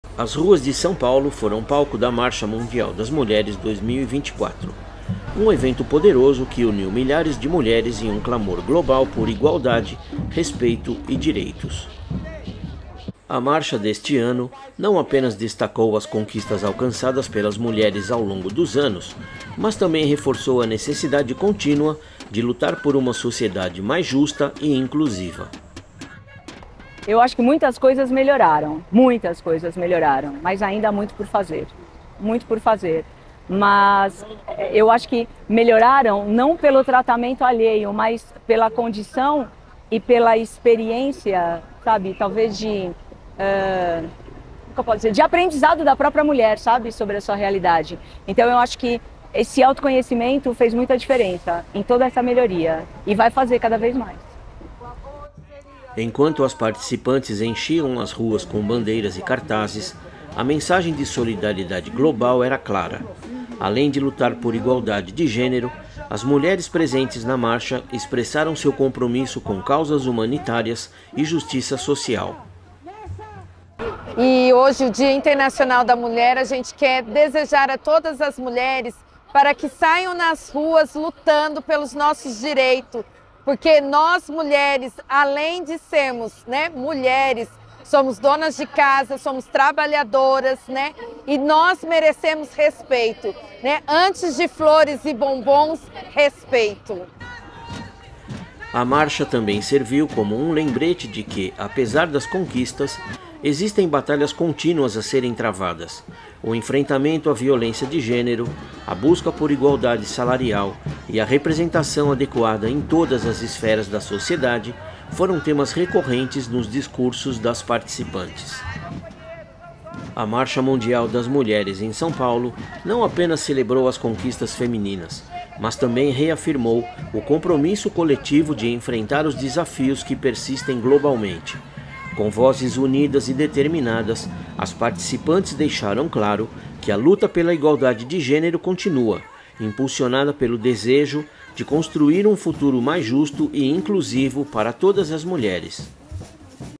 Marcha Mundial das Mulheres 2024
As ruas de São Paulo foram palco da Marcha Mundial das Mulheres 2024, um evento poderoso que uniu milhares de mulheres em um clamor global por igualdade, respeito e direitos.

A marcha deste ano não apenas destacou as conquistas alcançadas pelas mulheres ao longo dos anos, mas também reforçou a necessidade contínua de lutar por uma sociedade mais justa e inclusiva.

Enquanto as participantes enchiam as ruas com bandeiras e cartazes, a mensagem de solidariedade global era clara. Além de lutar por igualdade de gênero, as mulheres presentes na marcha expressaram seu compromisso com causas humanitárias e justiça social.